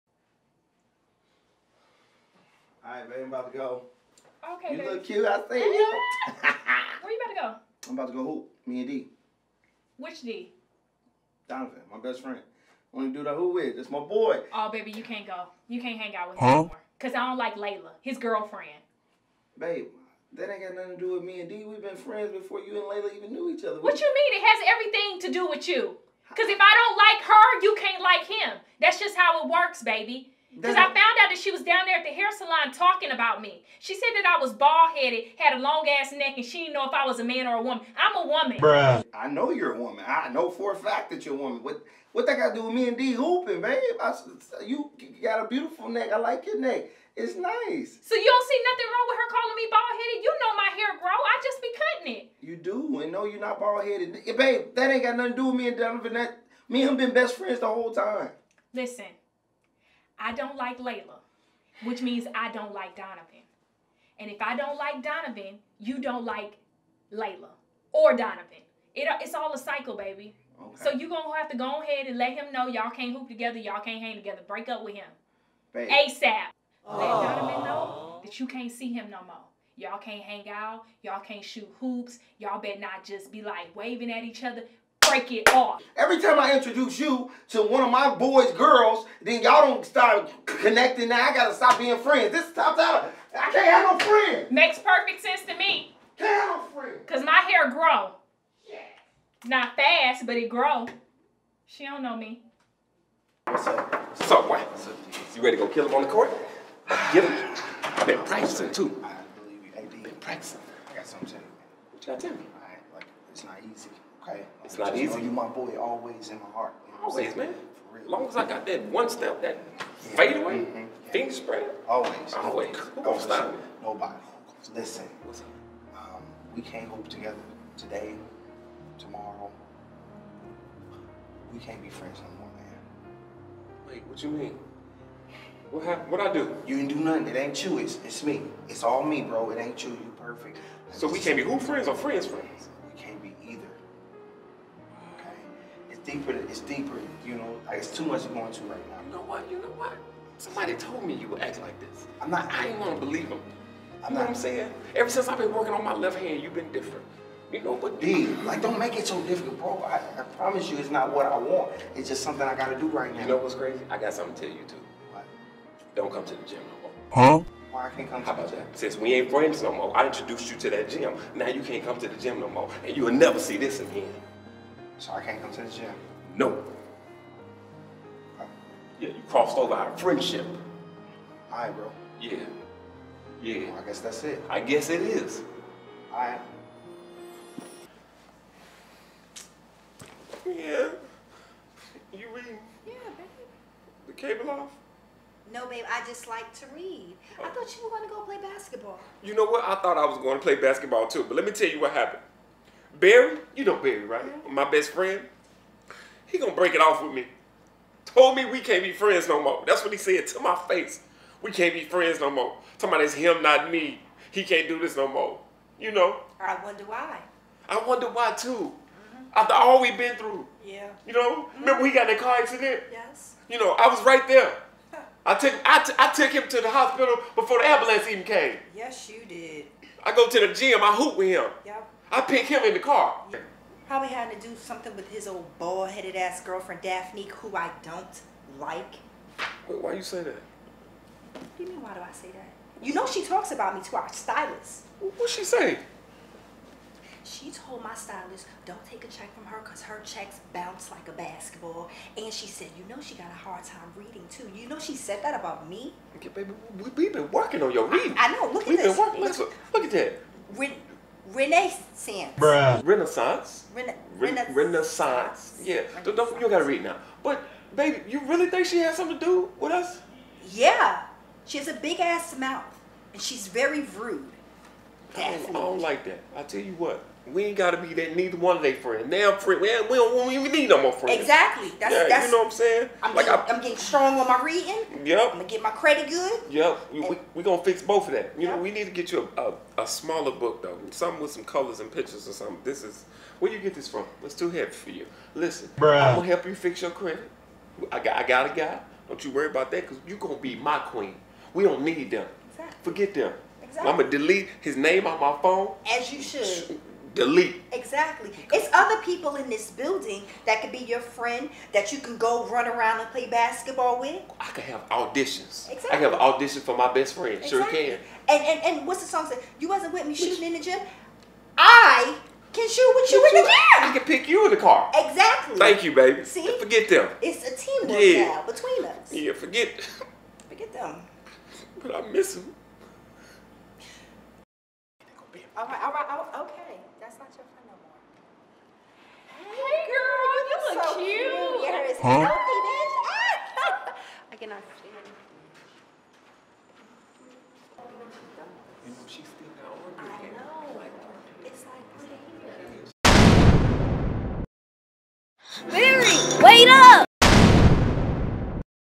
Best Friends Chronicles Pt. 1
When your girl and your homeboy's girl are at odds.